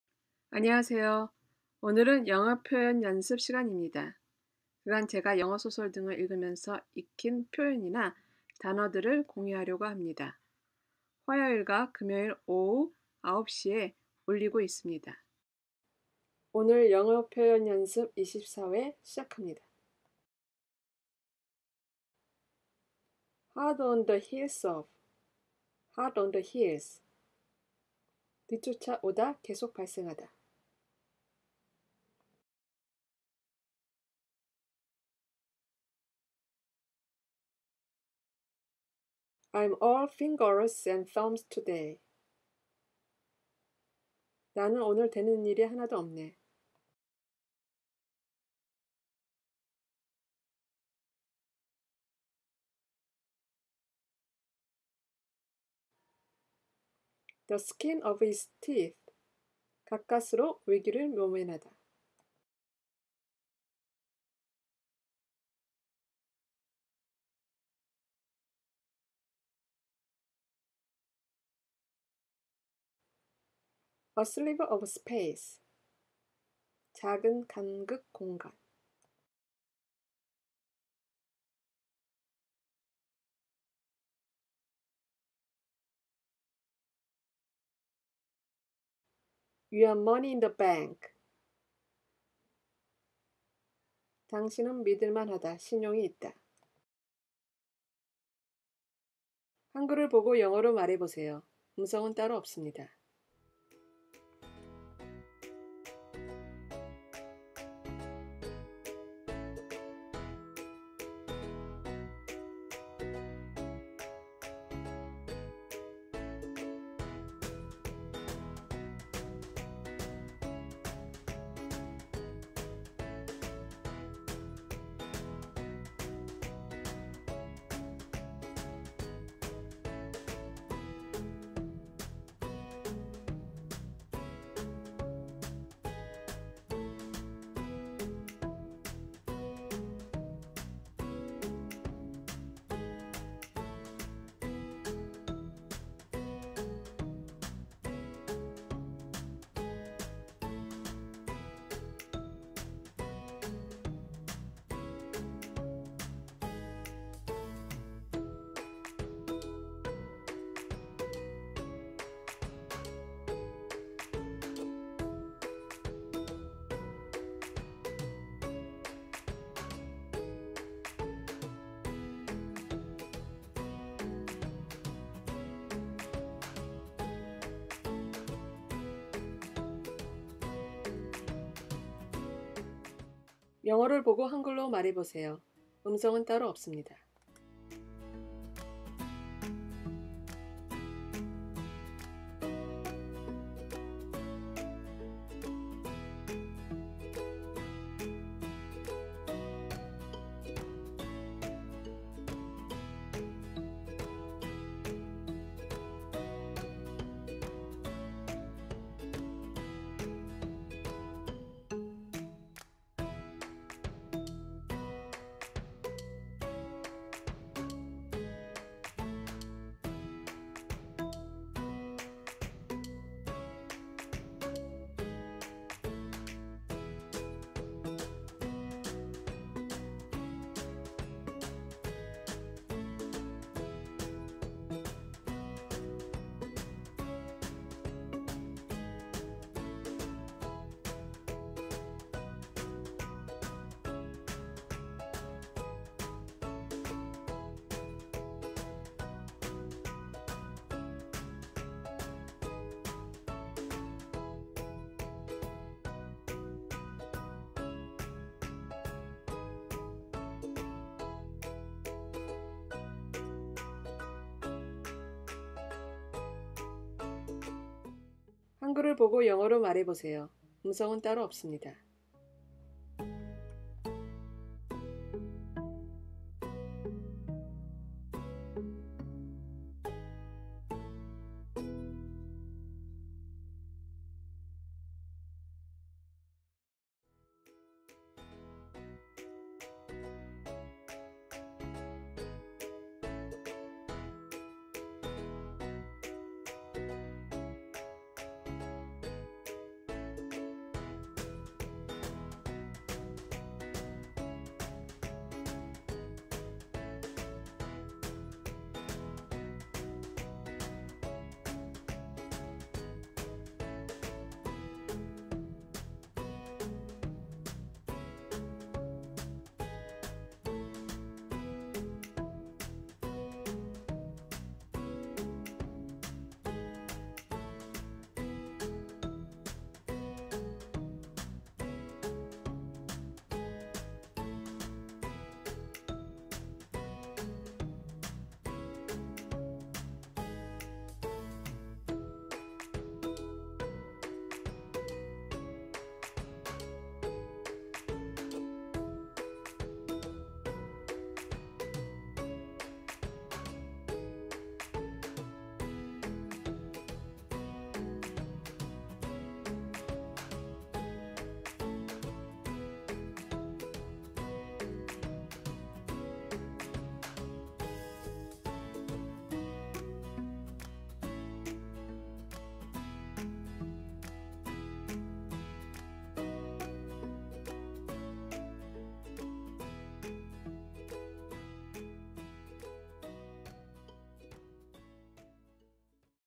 [영어표현연습 24] A sliver of space 작은 간극
안녕하세요?
그간 제가 영어소설등을 읽으면서 적어둔 표현이나 문장등을 같이 공유해볼까 합니다.
지금 계획은 한번에 5개씩 일주일에 두번 올려보려고 합니다.
화,금 오후 9시에 올리려고 합니다.
이건 강의가 아니고요 연습자료입니다. 영어발음도 좋지 않고 오류도 있을수 있습니다.
오늘은  24회입니다.

1:40 한영연습
3:07 영한연습
4:30 한영연습

* Hard on the heels of /Hot on the heels
* I’m all fingers and thumbs today.
* The skin of his teeth
* A sliver of space
* You are money in the bank.